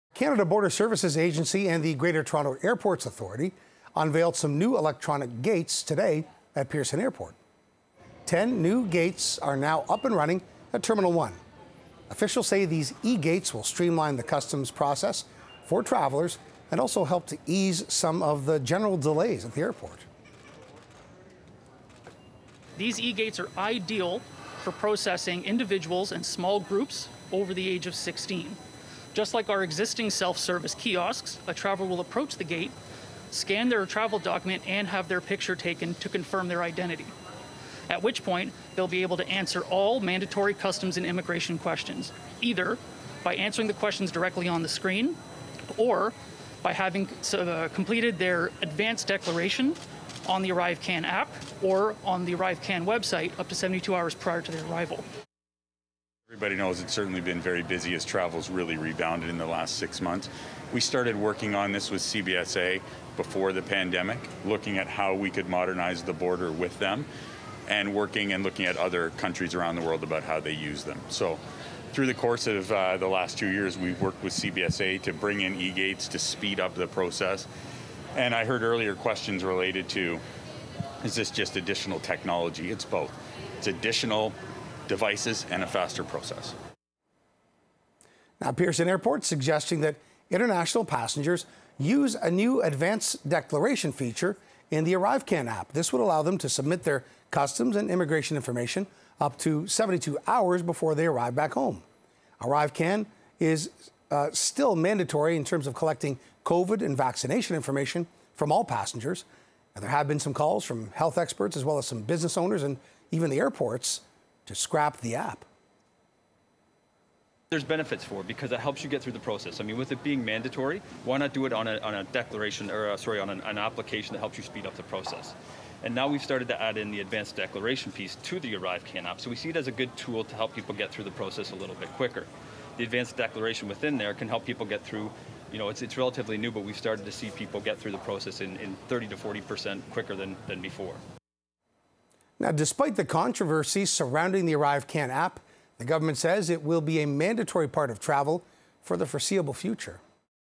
New electronic gates at Pearson airport
New electronic gates were unveiled at Pearson Airport today, in a effort to make getting through the airport faster and more efficient.